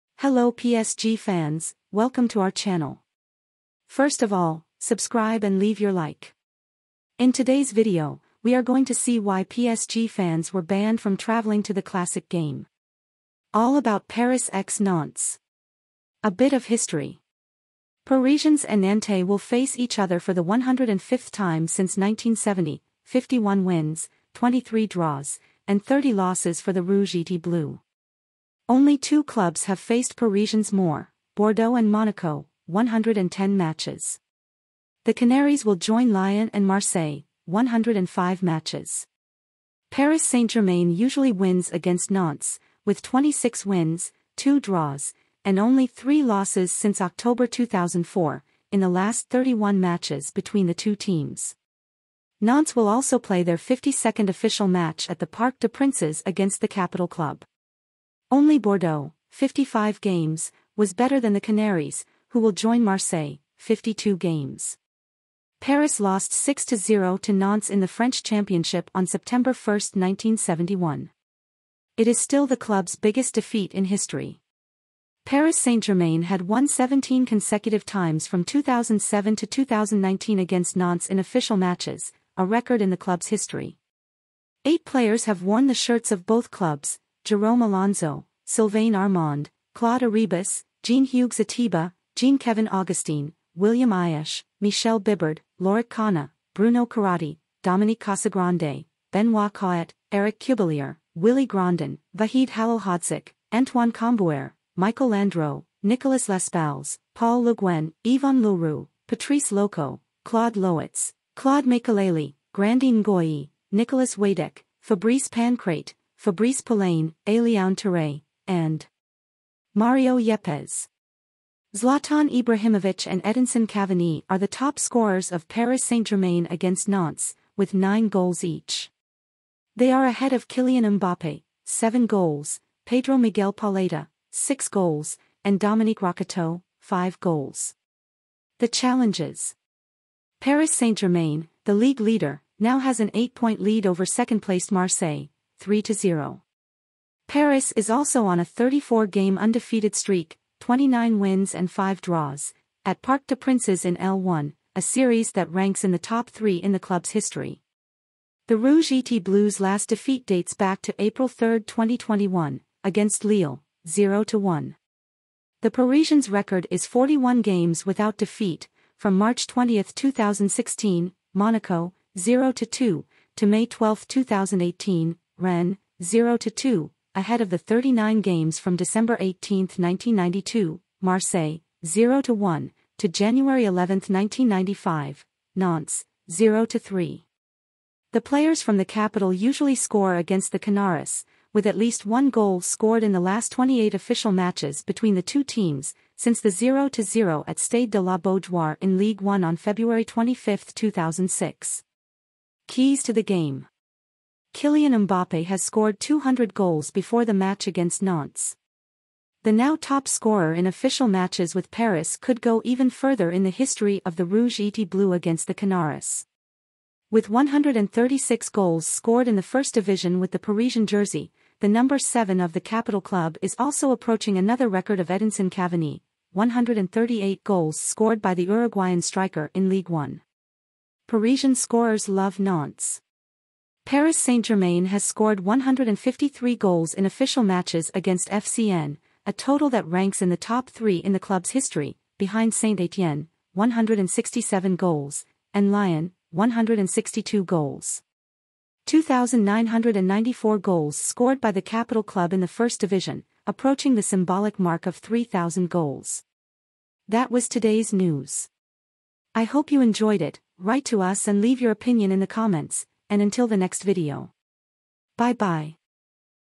PSG X NANTES
In today's video, we will see why the PSG fans were banned from traveling to the classic match; all about Paris x Nantes!